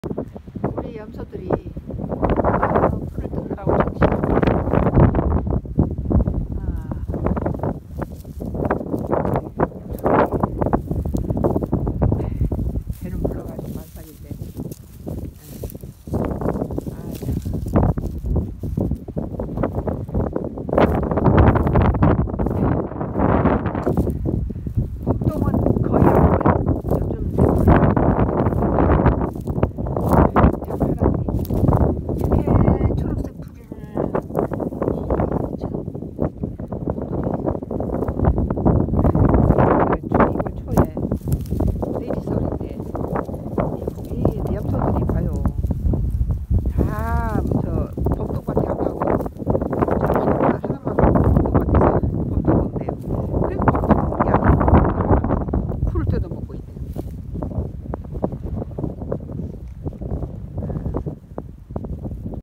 명절 전날도~~~우리집 염소들은 새 풀밭에 진수성찬~~~~!@sungodcross
명절 전날도~~~우리집 염소들은 새 풀밭에 진수성찬 !

요즘 남쪽은 풀밭에 새순이 돋아서~~~!
내일이 구정인데~~~우리염소들은 풀밭에서 새풀로 !
진수성찬 잔치를 벌렸네~~~!@sungodcross